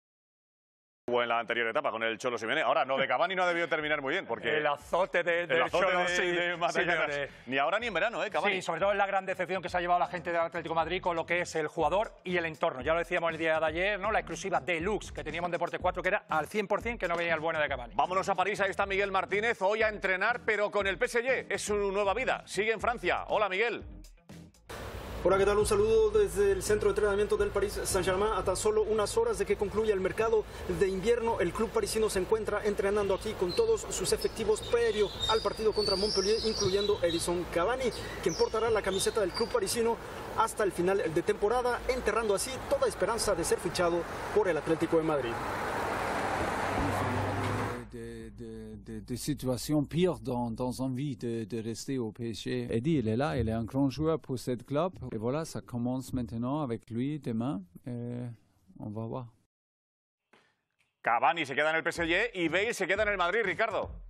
CAVANI NO SE VA DEL PSG
24 horas después de los rumores ante la salida de Cavani del PSG, todas las negociaciones no lograron su objetivo y el jugador uruguayo se quedará en el PSG